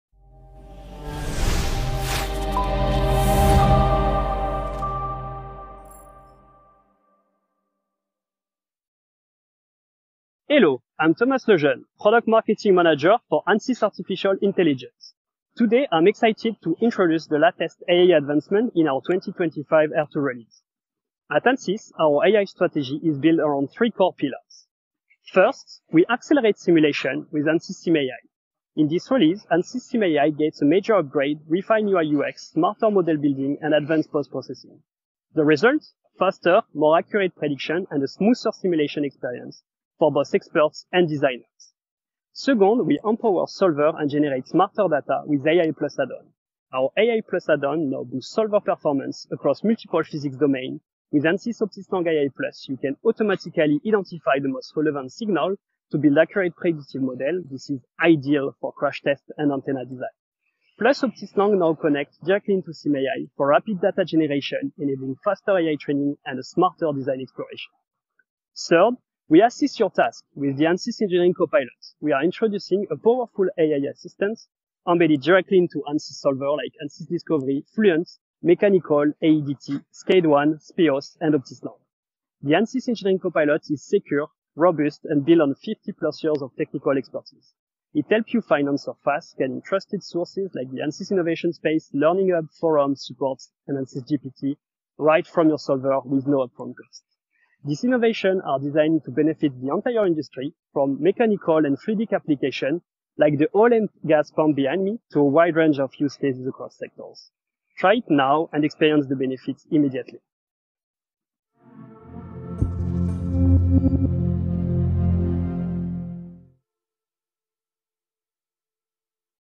What's New with Ansys AI | Ansys 2025 R2 | Smarter Simulation with SimAI, AI+ & Engineering Copilot
Get ready to redefine your simulation workflows with the latest AI advancements in Ansys 2025 R2. In this release overview, Thomas Lejeune, Product Marketing Manager for Ansys AI, walks you through the three pillars of Ansys’ next-gen AI strategy — helping engineers simulate smarter, faster, and more accurately than ever.

With powerful upgrades to SimAI, solver-boosting AI+ Add-ons, and the intuitive Ansys Engineering Copilot, this release brings enterprise-ready AI into the heart of your design process — across mechanical, fluids, electronics, and systems.

 AI Innovations in 2025 R2:
- SimAI: Now features refined UI/UX, faster model building, and smarter post-processing
- AI+ Add-ons: Boost solver performance across physics with automated signal detection (ideal for crash, RF, & more)
- SimAI + AI+ integration: Seamless link for rapid data generation and AI training
- Ansys Engineering Copilot: A built-in AI assistant in tools like Fluent, Discovery, Mechanical, AEDT, Scade One, and more
- Copilot connects to trusted resources: AnsysGPT, Innovation Space, Learning Hub, and Forums

Whether you’re optimizing pump designs, simulating crash safety, or scaling antenna systems, Ansys AI tools help you deliver innovation faster and smarter.

00:00 Introduction to Ansys 2025 R2 AI Advancements
00:18 Ansys SimAI Upgrades: UI, Speed & Prediction
00:42 Ansys AI+ Add-ons for Solver Acceleration
01:05 Use Cases: Crash Tests & Antenna Design
01:18 SimAI & AI+ Integration for Rapid AI Training
01:29 Ansys Engineering Copilot: Your In-Tool AI Assistant